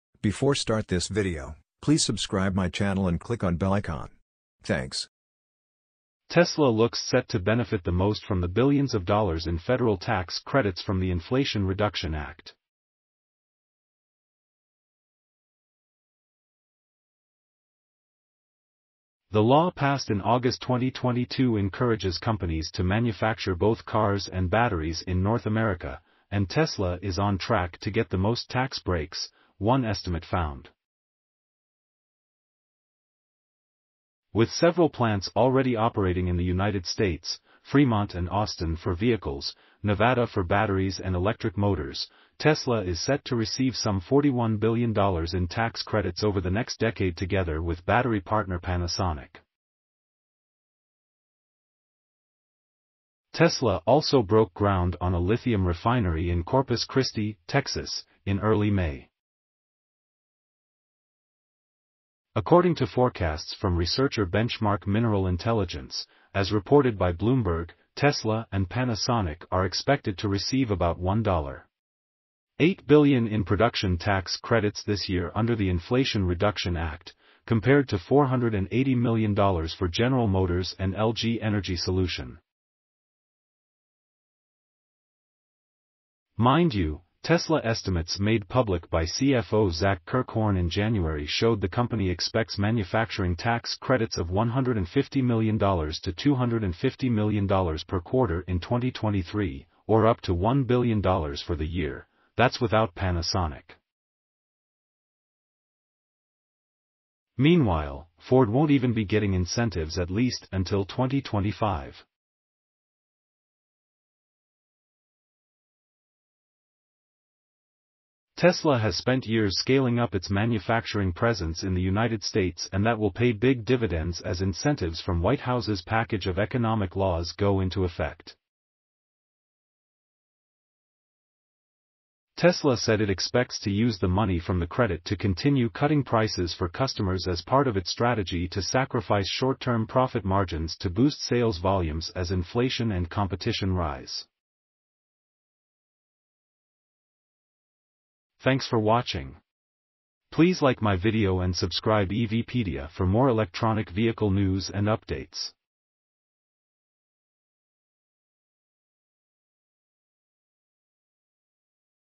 Tesla And Panasonic Could Get $41 Billion In Tax Credits By 2032
Tesla looks set to benefit the most from the billions of dollars in federal tax credits from the Inflation Reduction Act. The law passed in August 2022 encourages companies to manufacture both cars and batteries in North America, and Tesla is on track to get the most tax breaks, one estimate found. With several plants already operating in the United States – Fremont and Austin for vehicles, Nevada for batteries and electric motors – Tesla is set to receive some $41 billion in tax credits over the next decade together with battery partner Panasonic. Tesla also broke ground on a ...